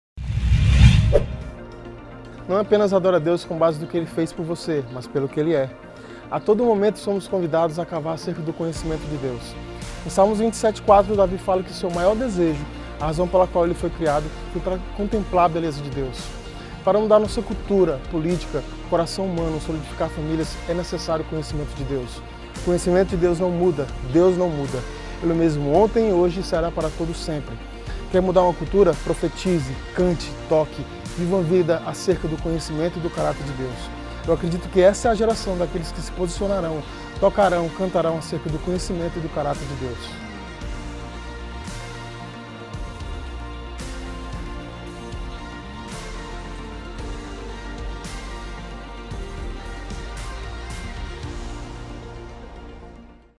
Vivendo para Conhecer a Deus | Fogo Rápido | Killdely Melo
Nós podemos mudar a nossa realidade a partir das verdades imutáveis do caráter de Deus. Escolha viver para conhecer a Deus.

Uma simples faísca pode iniciar uma grande explosão. FOGO RÁPIDO traz reflexões curtas e práticas de 1 minuto queremos te impulsionar a queimar mais por Jesus.

Siga @fhopbr no instagram!